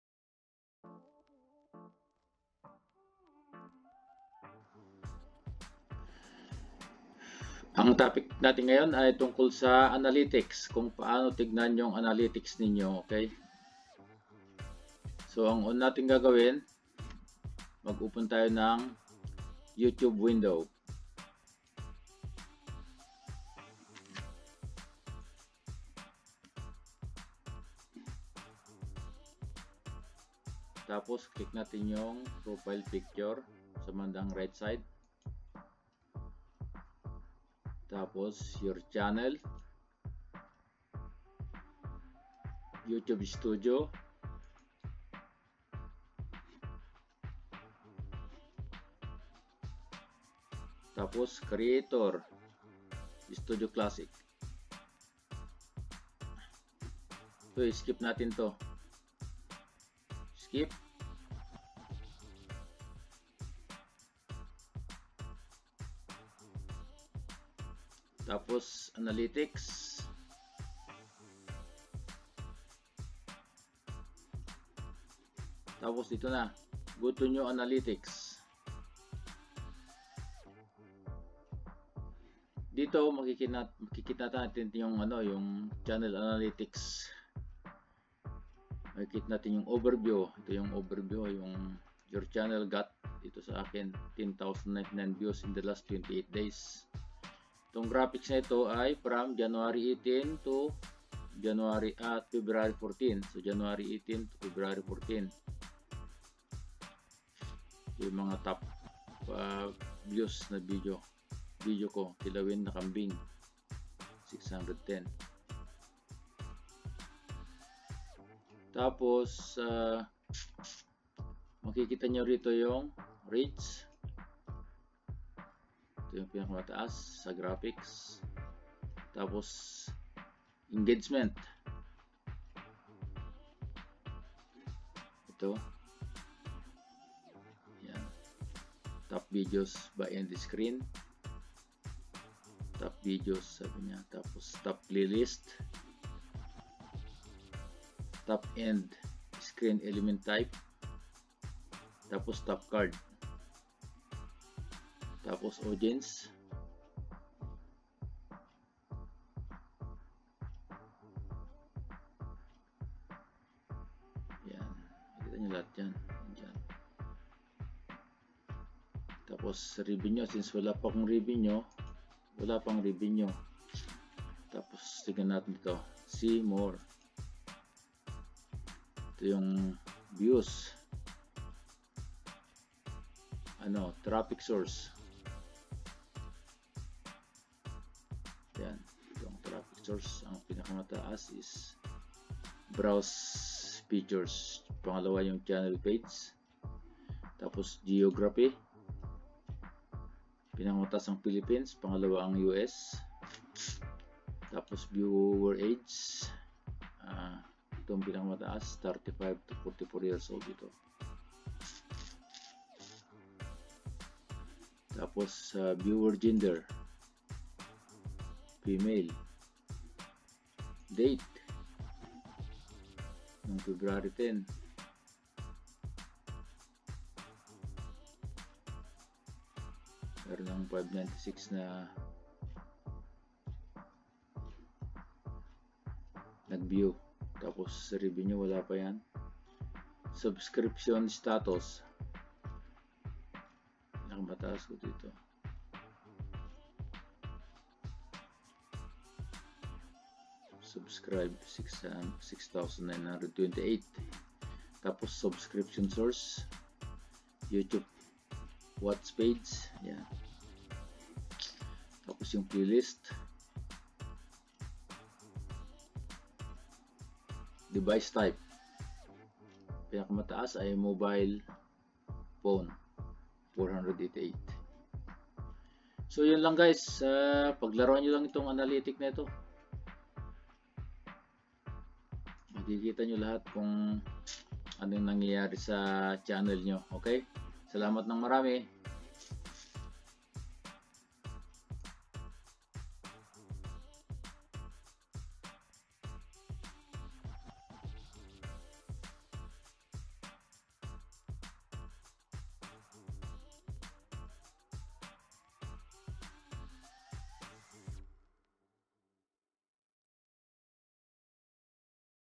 How to check your analytics
Tutorial on how to check the activities of your channel